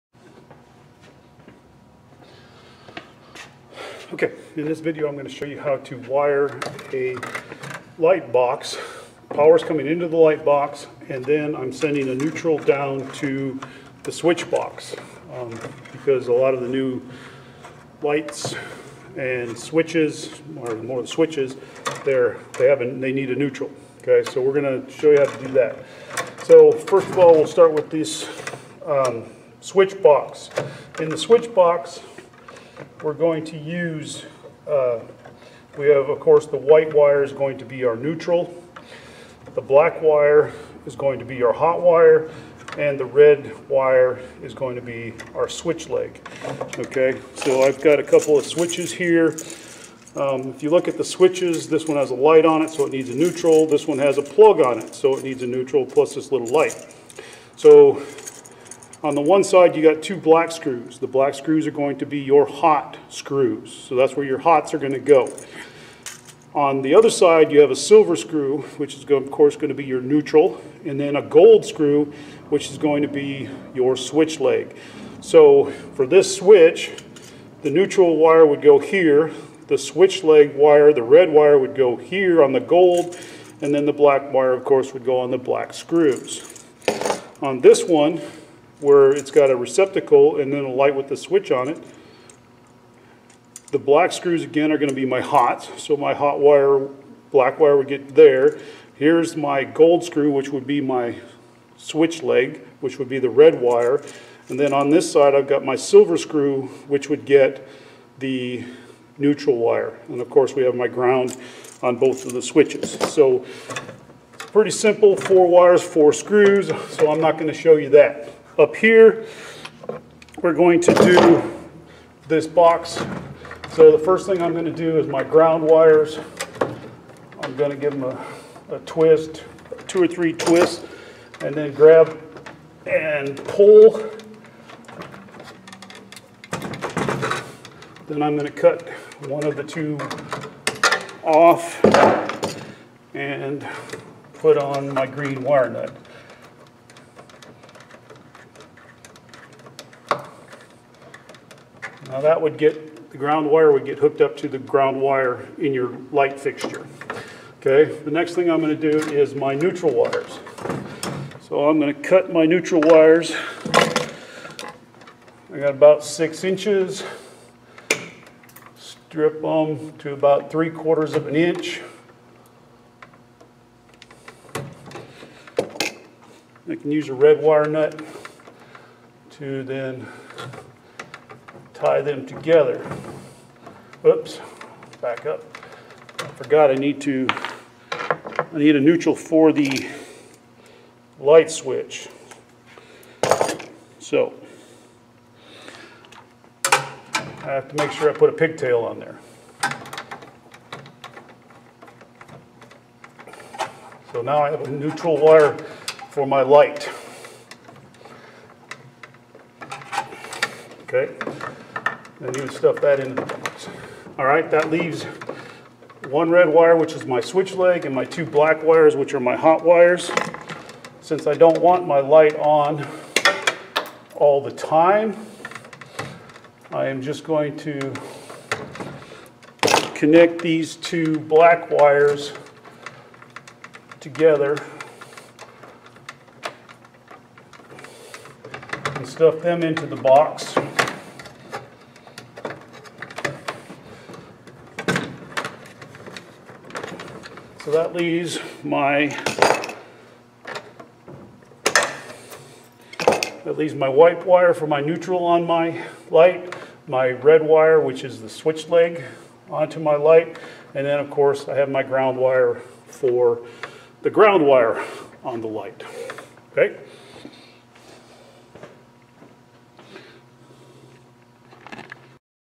Power to light, neutral to switch
In this video I demonstrate how to wire a light with the power wire running into the light box and having a neutral wire in the switch box.